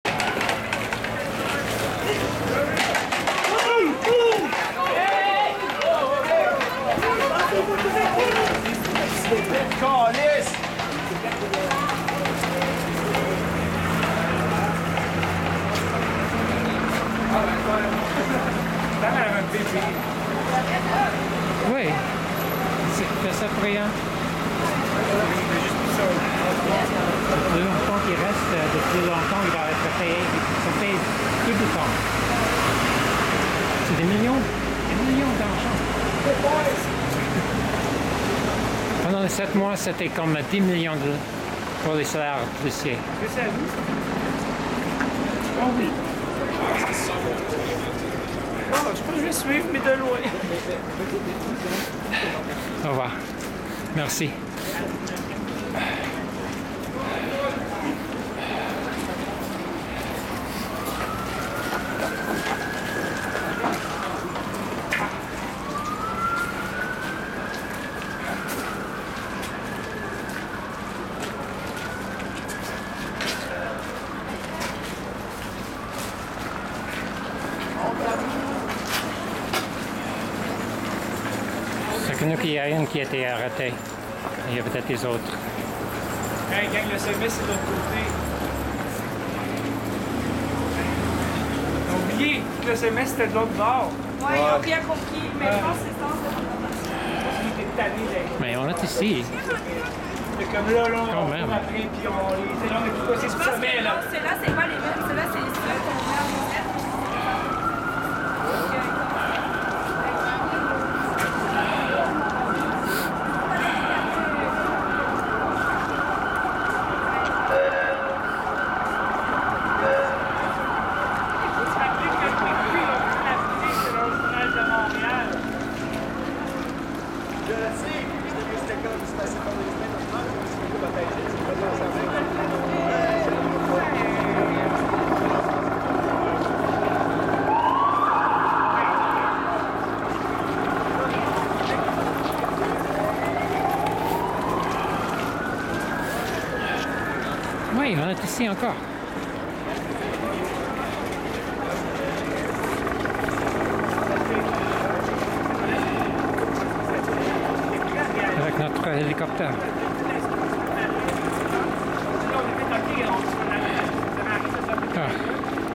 24/24 - 2013-02-25 Manif pour la gratuité scolaire Montréal
Première manifestation de l'année qui déclenche une avalanche. Pour le gratuité scolaire.

First demo of the year to set off an avalanche of protests. The SPVM political police tried to stop it with a municipal regulation P-6, and resorting to using the 2.1 sub-regulations to declare the May 1 demo a danger to the peace, i.e. revolutionary - for having marched towards the bourgeois-mafia club '357C'.